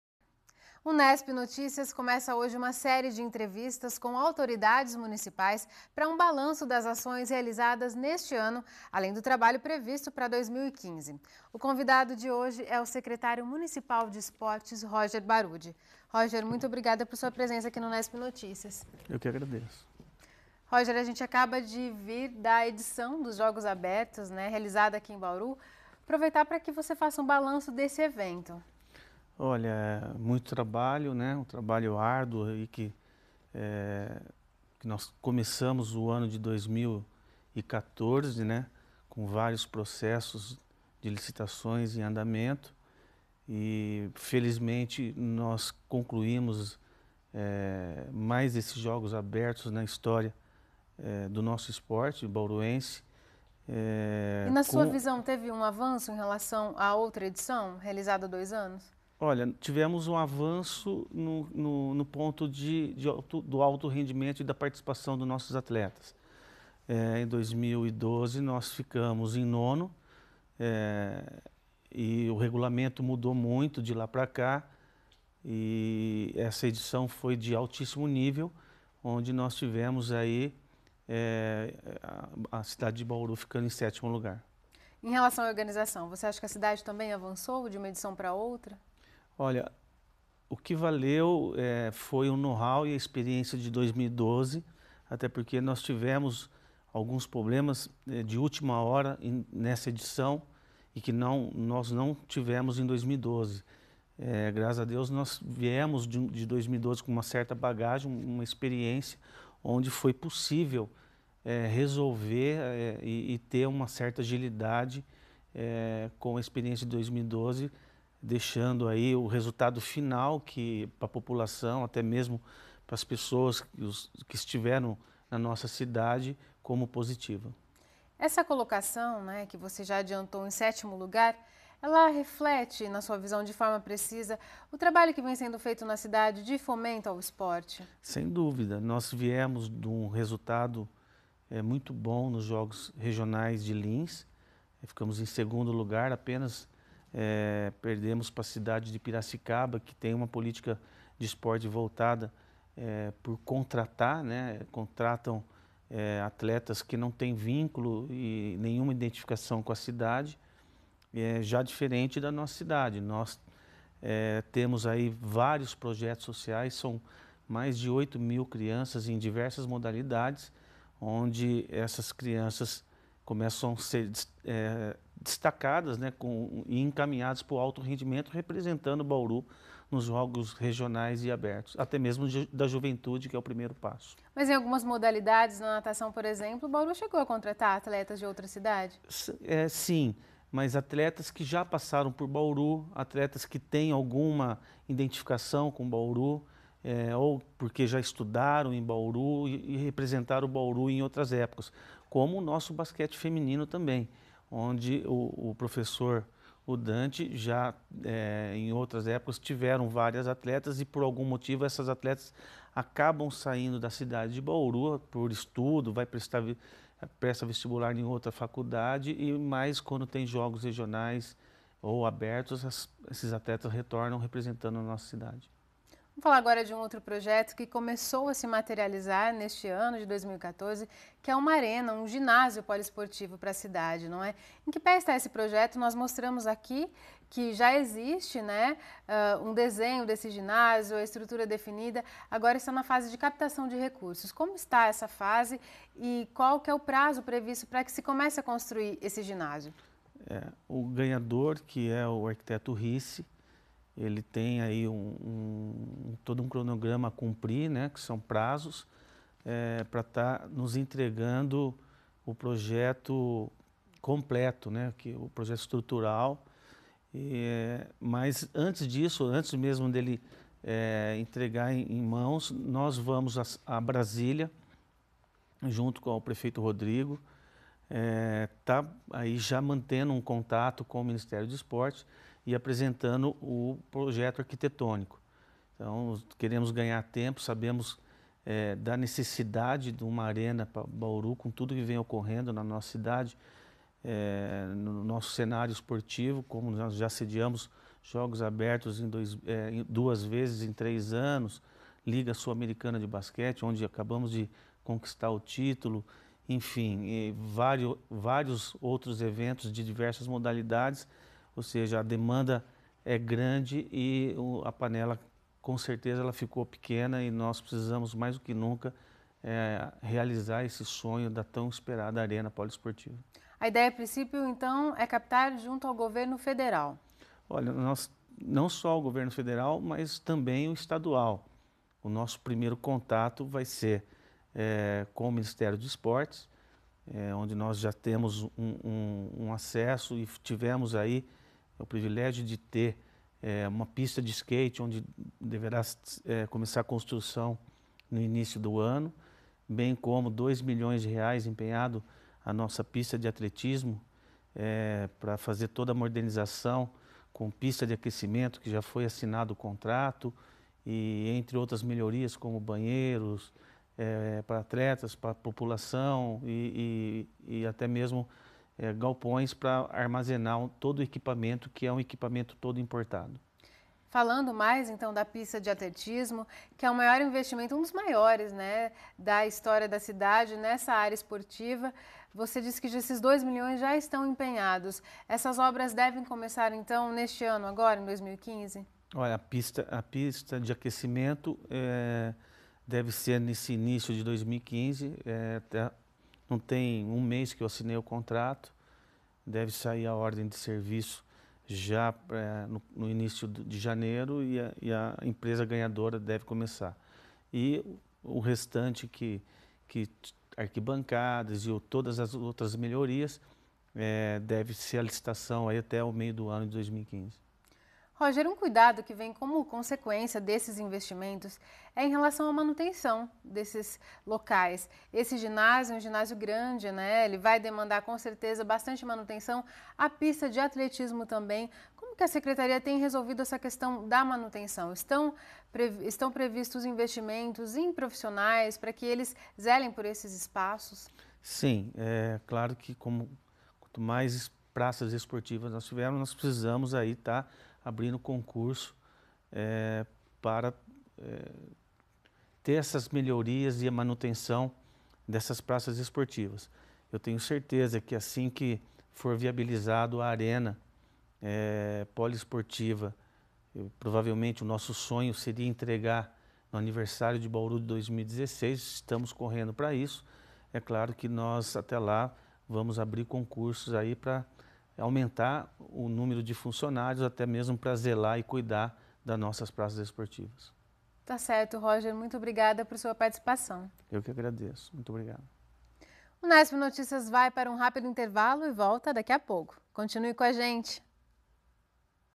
Unesp Notícias - 22/12/2014 - Barude inicia série de entrevistas com secretários de Bauru - M
O Unesp Notícias inicia nesta segunda-feira (22/12) uma série de entrevistas com secretários municipais. O objetivo é que os titulares façam uma avaliação do trabalho desenvolvido ao longo de 2014 e projetem o ano que vem. Roger Barude, responsável pela Semel (Secretaria Municipal de Esportes e Lazer), comenta, entre outros assuntos, sobre a participação de Bauru nos Jogos Abertos deste ano e a possibilidade de a cidade ganhar um novo ginásio até 2016.